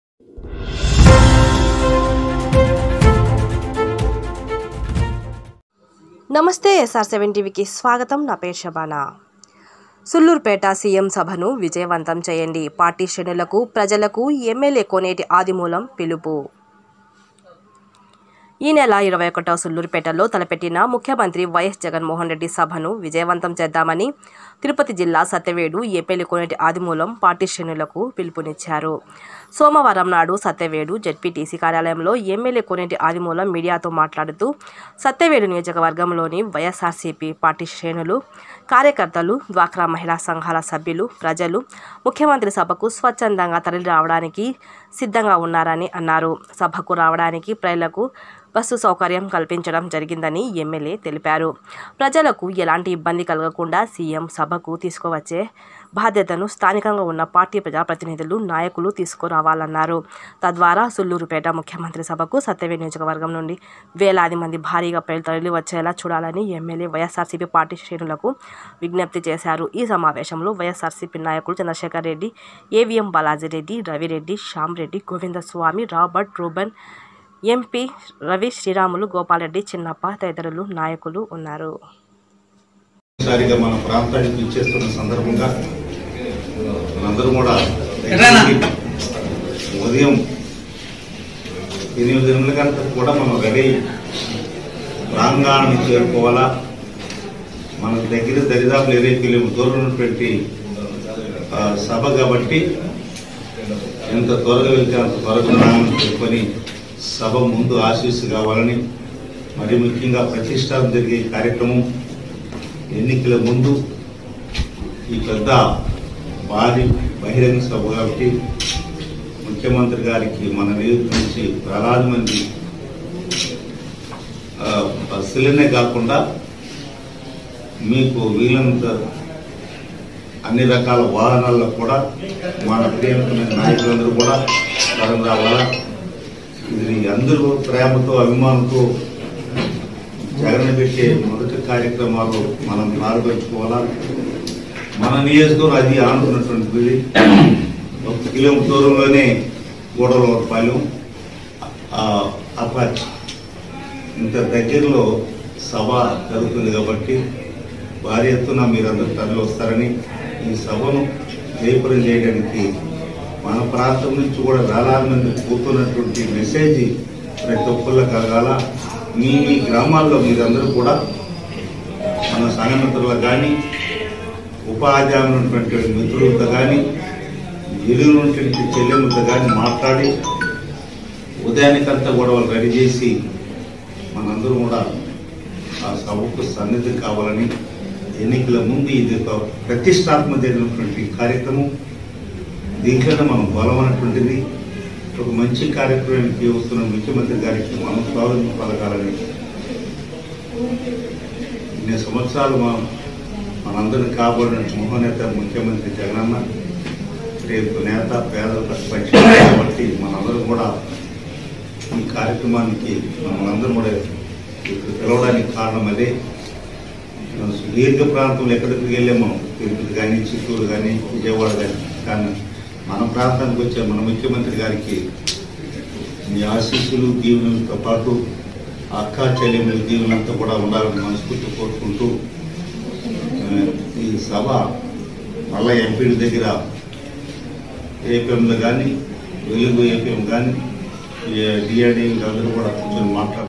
సూళ్లూరుపేట సీఎం సభను విజయవంతం చేయండి.ఎమ్మెల్యే కోనేటి ఆదిమూలం..
సూళ్లూరుపేట సీఎం సభను విజయవంతం చేయండి.

పార్టీ శ్రేణులకు, ప్రజలకు, ఎమ్మెల్యే కోనేటి ఆదిమూలం పిలుపు*

ఈనెల 21న సూళ్లూరుపేటలో తలపెట్టిన ముఖ్యమంత్రి వైయస్ జగన్మోహన్ రెడ్డి సభను విజయవంతం చేద్దామని తిరుపతి జిల్లా ..సత్యవేడు ఎమ్మెల్యే కోనేటి ఆదిమూలం పార్టీ శ్రేణులకు, ప్రజలకు పిలుపునిచ్చారు.

సోమవారం నాడు సత్యవేడు జడ్పిటిసి  కార్యాలయంలో ఎమ్మెల్యే  కోనేటి ఆదిమూలం మీడియాతో మాట్లాడుతూ  సత్యవేడు నియోజకవర్గంలోని వైఎస్ఆర్సిపి పార్టీ శ్రేణులు కార్యకర్తలు డ్వాక్రా మహిళ సంఘాల సభ్యులు, ప్రజలు ముఖ్యమంత్రి సభకు స్వచ్ఛందంగా తరలిరావడానికి సిద్ధంగా ఉన్నారని అన్నారు.

సభకు రావడానికి ప్రజలకు బస్సు సౌకర్యం కల్పించడం జరిగిందని ఎమ్మెల్యే తెలిపారు.

ప్రజలకు ఎలాంటి ఇబ్బంది కలగకుండా సీఎం సభకు తీసుకువచ్చే బాధ్యతను స్థానికంగా ఉన్న పార్టీ ప్రజాప్రతినిధులు, నాయకులు తీసుకోవాలన్నారు.

తద్వారా సూళ్లూరుపేట ముఖ్యమంత్రి సభకు సత్యవేడు నియోజకవర్గం నుండి వేలాదిమంది భారీగా ప్రజలు తరలివచ్చేలా చూడాలని ఎమ్మెల్యే వైయస్ఆర్సీపీ పార్టీ శ్రేణులకు విజ్ఞప్తి చేశారు.

ఈ సమావేశంలో వైఎస్ఆర్సిపి నాయకులు చంద్రశేఖర్ రెడ్డి ఏవీఎం బాలాజీ రెడ్డి రవి రెడ్డి శ్యామ్ రెడ్డి గోవిందస్వామి రాబర్ట్ రూబన్ ఎంపీ రవి శ్రీరాములు గోపాల్ రెడ్డి చిన్నప్ప తదితర నాయకులు ఉన్నారు.